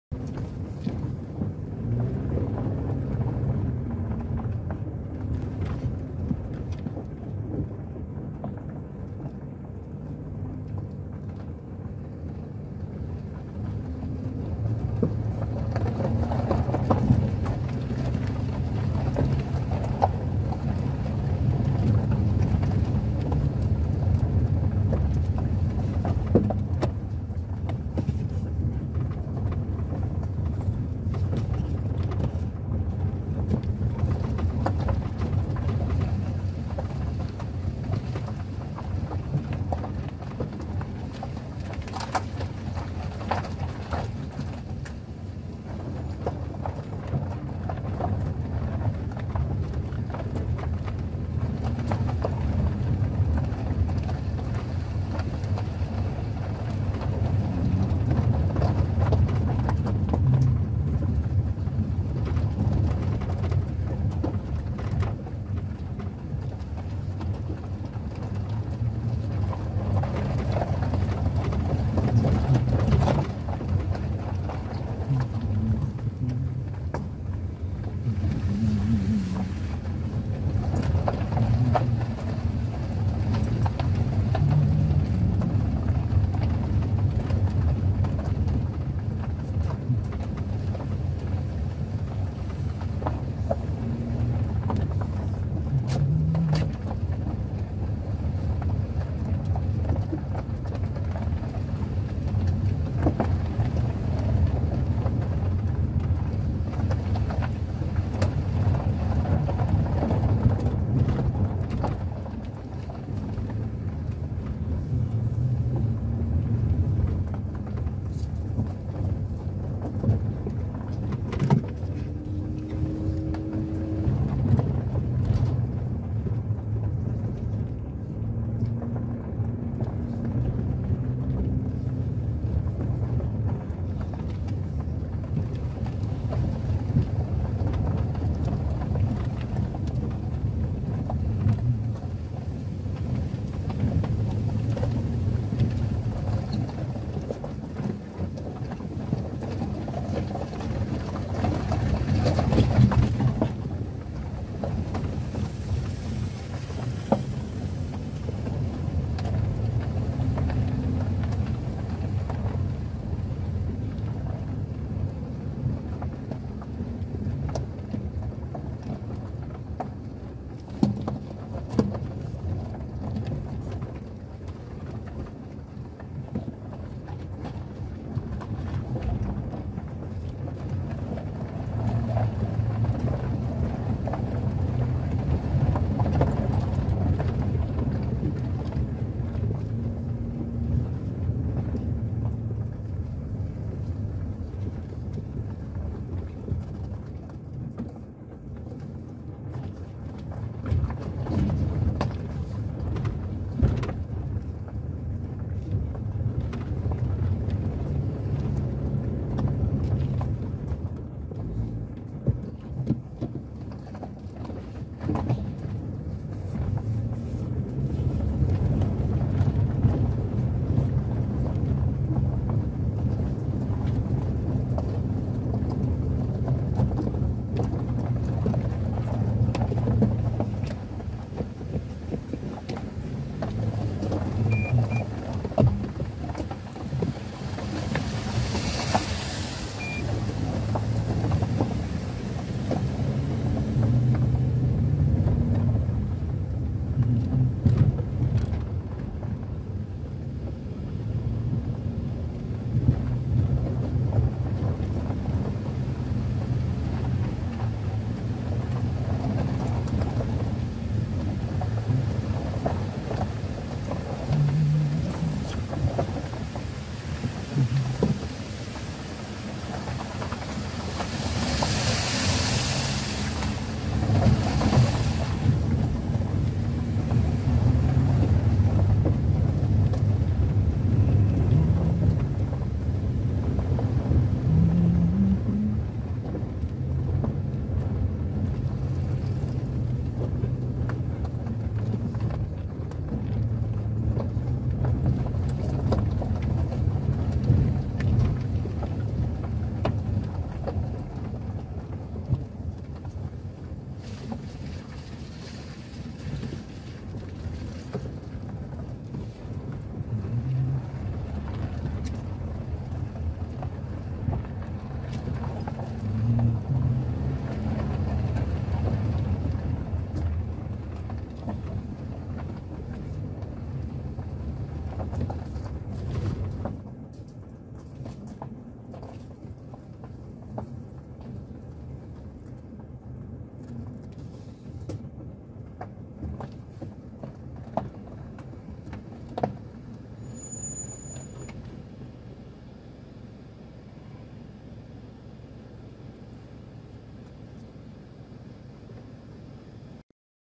Roadtrip Valais Switzerland
fun drive on steep narrow road in central Valais, Switzerland